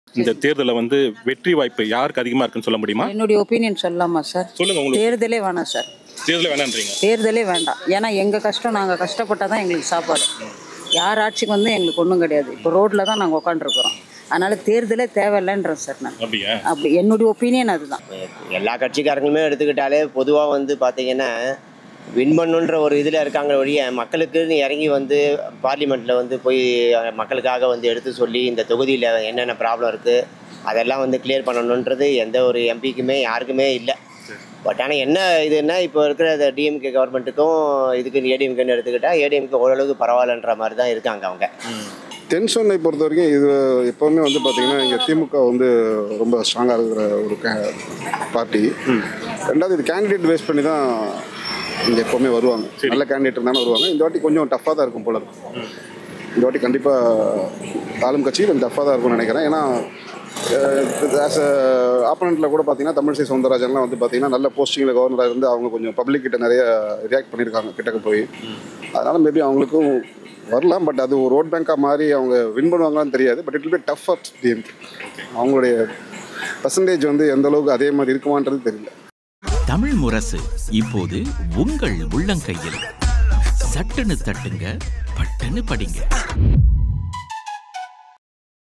தென்சென்னையில் வாகை சூடப்போவது யார்
தென்சென்னையில் வாகை சூடப்போவது யார்? வாக்காளர்கள் கருத்து